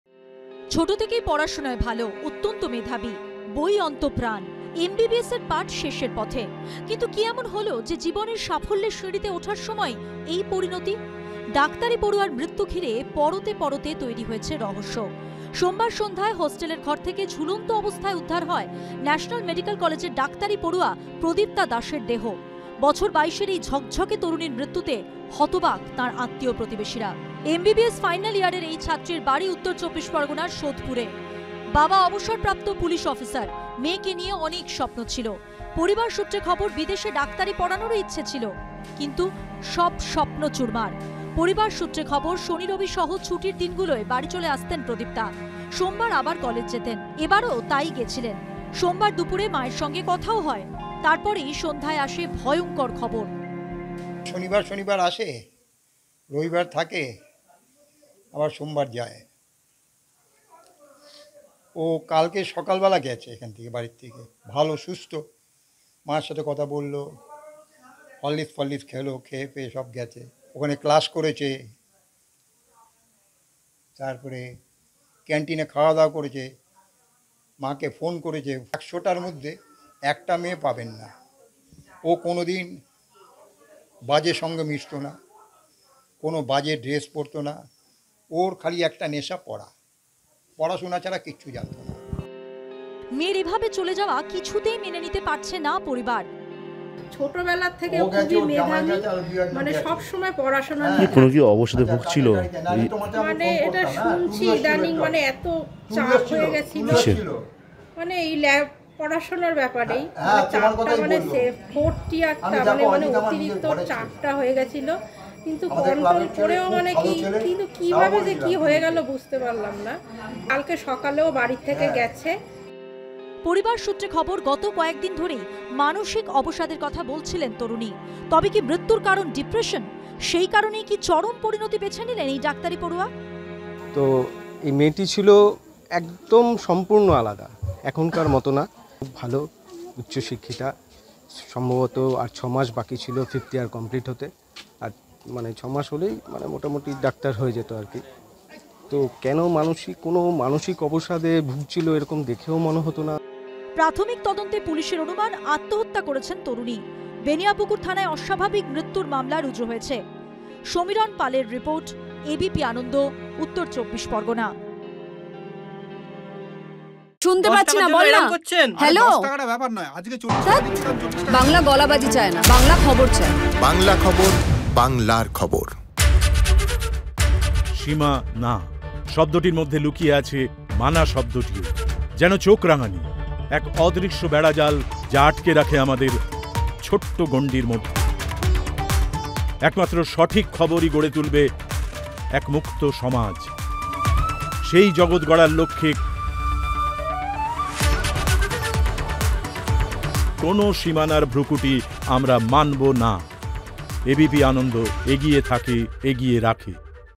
MBBS Student Death : অবসাদের জেরেই কি চরম সিদ্ধান্ত ডাক্তারি পড়ুয়া প্রদীপ্তা দাসের ?
অবসাদের জেরেই কি চরম সিদ্ধান্ত ? কিন্তু কী এমন ঘটল, যার জেরে, ২২ বছরের ঝকঝকে জীবনে ইতি টানলেন ডাক্তারি পড়ুয়া? ন্যাশনাল মেডিক্যাল কলেজের MBBS ফাইনাল ইয়ারের ছাত্রীর মৃত্যুর খবরে হতবাক তাঁর আত্মীয় পরিজনরা।